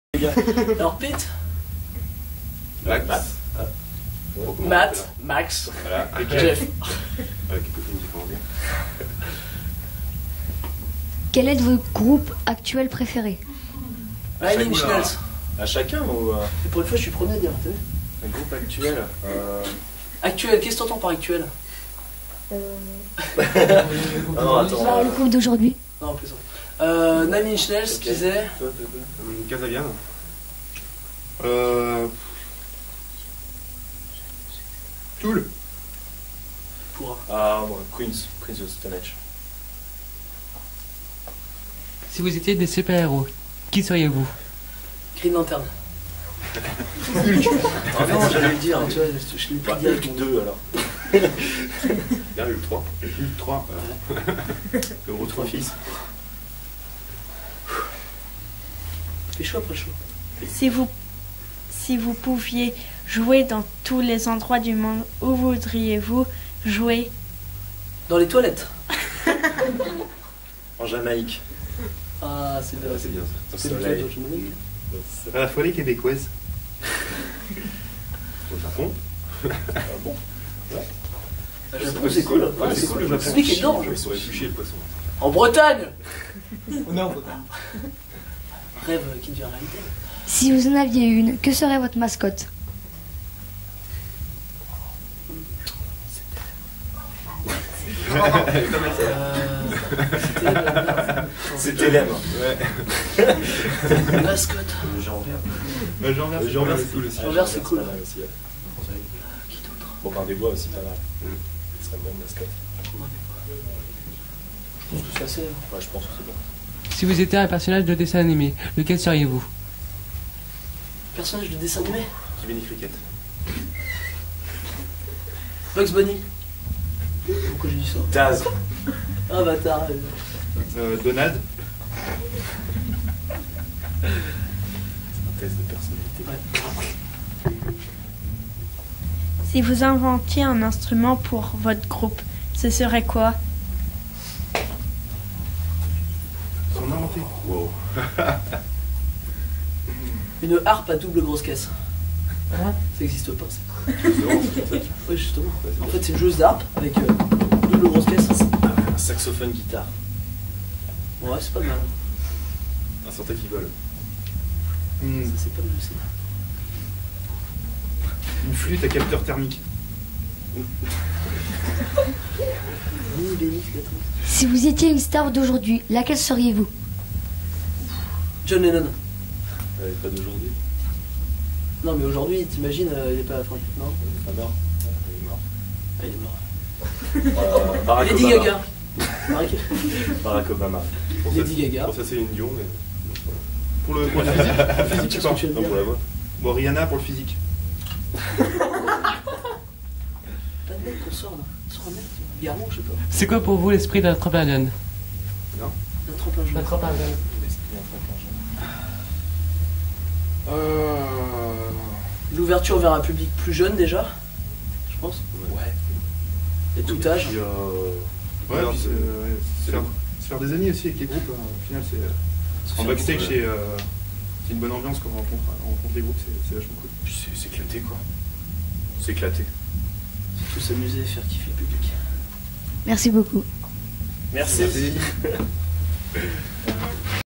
Toshes Interview finale Tremplin
Interview du groupe Toshes

Finaliste des Tremplin jeunes 2015 - ils termineront en 3ème place